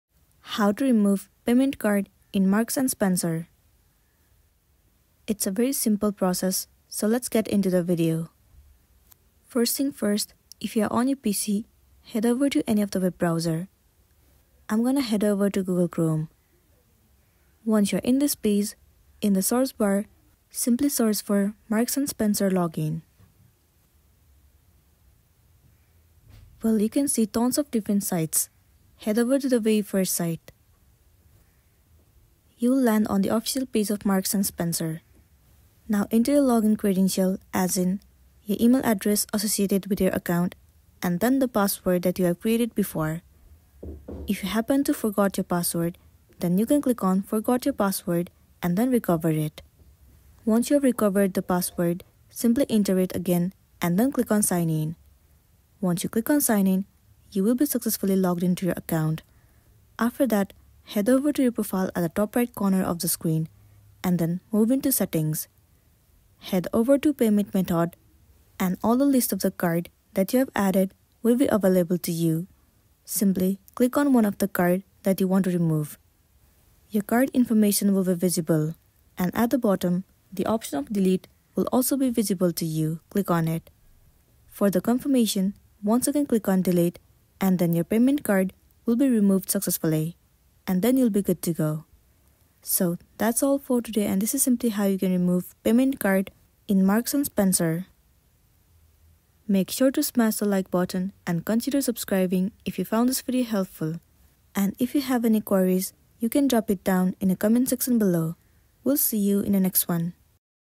How to Remove Payment Card on Marks & Spencer (How do I cancel a payment on my M&S credit card?)
How to Remove a Payment Card on Marks & Spencer | How Do I Cancel a Payment on My M&S Credit Card?

Need to update your payment options or cancel a transaction on your M&S credit card? Here’s what you need to know for 2024:

In this guide:
0:00 - Introduction
0:05 - How to Remove a Payment Card on Marks & Spencer

Removing a payment card from your Marks & Spencer account or canceling a payment involves accessing your account settings where you can manage your stored payment methods. For specific actions like canceling a credit card payment, it's best to contact Marks & Spencer's customer service directly for direct assistance to ensure your request is processed correctly and efficiently.

Manage your payment options smoothly at Marks & Spencer with these insights!